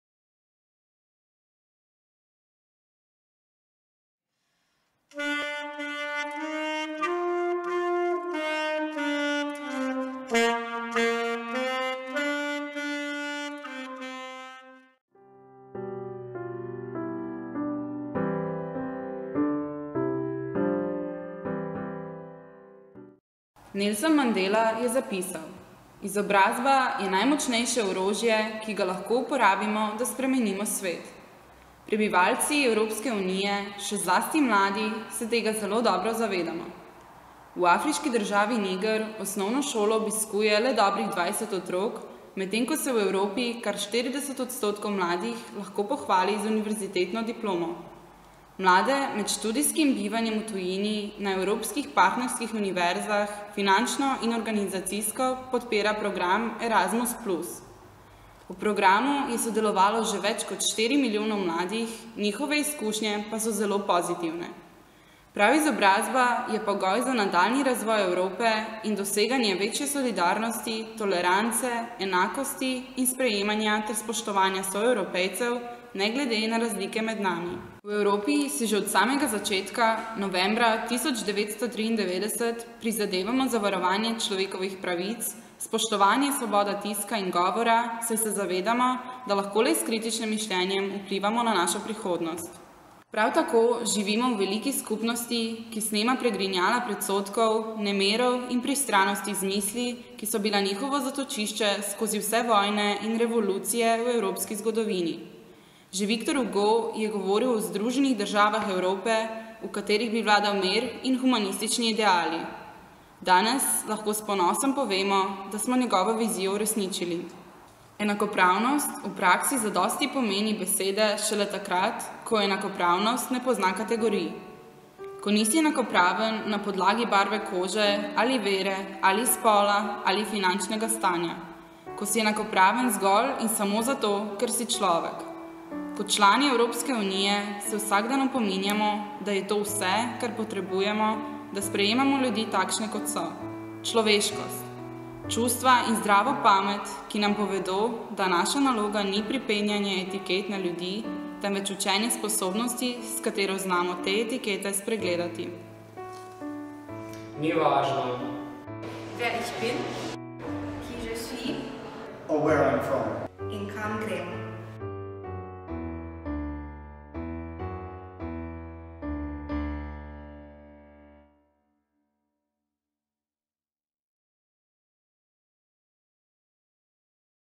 Ekipa PEGASUS-GimNM EVROŠOLA 2015
Avdiovizualni izdelek za projekt Evrošola 2015

Ekipa Gimnazije Novo mesto: PEGASUS

Montaža: Žiga Serini
Kamera: Tea Lajkovič
Besedilo: Anja Kvartuh in Tjaša Recelj

Člani ekipe: Eva Beg, Tisa Dobovšek, Marko Kaferle, Manja Krašovec, Anja Kvartuh, Tea Lajkovič, Sara Morosini, Nika Muren, Mateja Petaković, Eva Petruna, Ingrid Povše, Tjaša Recelj, Žiga Serini, Miha Tavčar.
Vse ostale osebe, ki nastopajo v filmu so profesorji in dijaki Gimnazije Novo mesto.